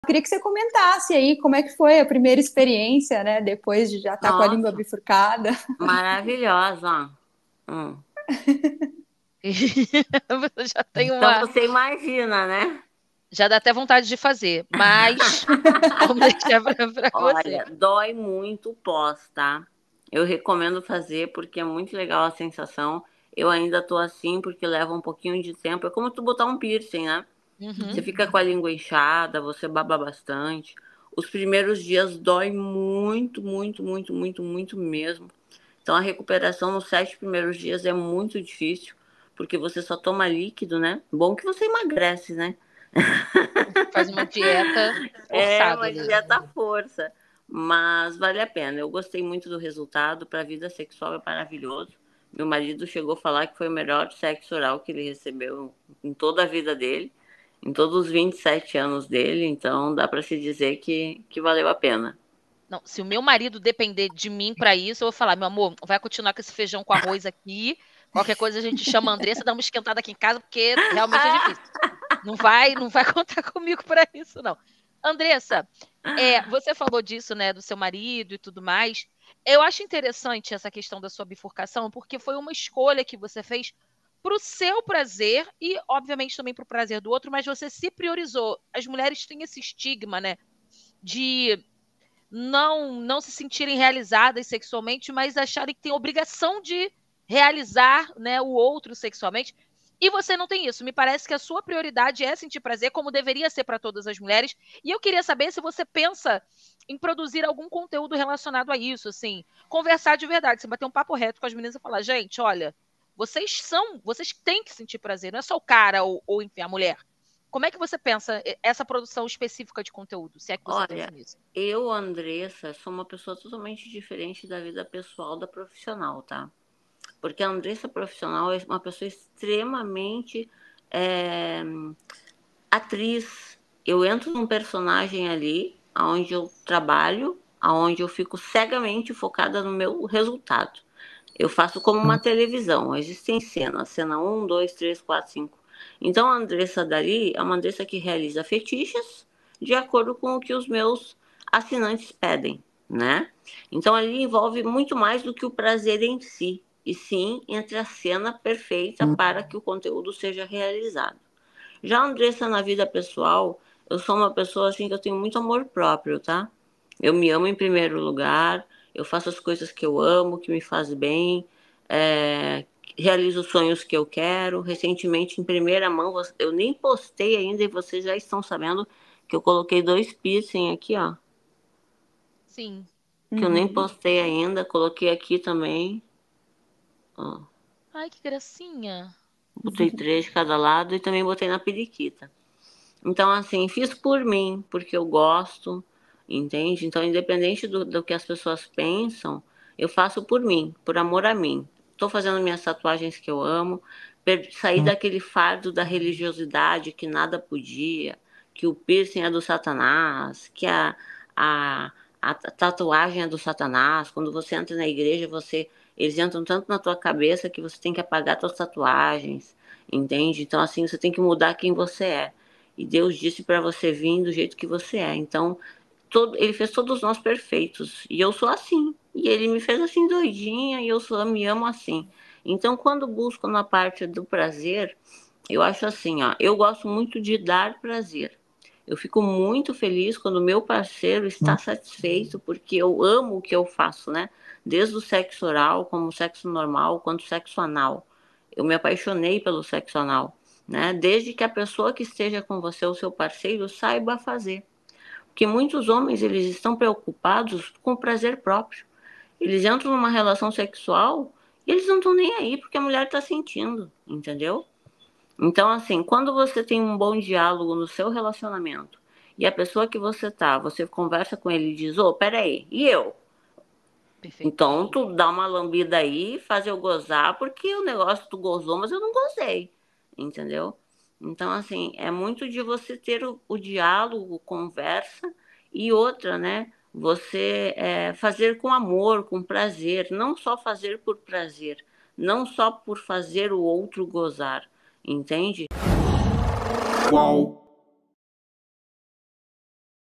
Sexo oral com língua bifurcada: Andressa Urach revela como foi reação do marido! Veja entrevista!
Andressa Urach contou como foi a reação do marido Lucas Matheus ao receber oral após língua bifurcada.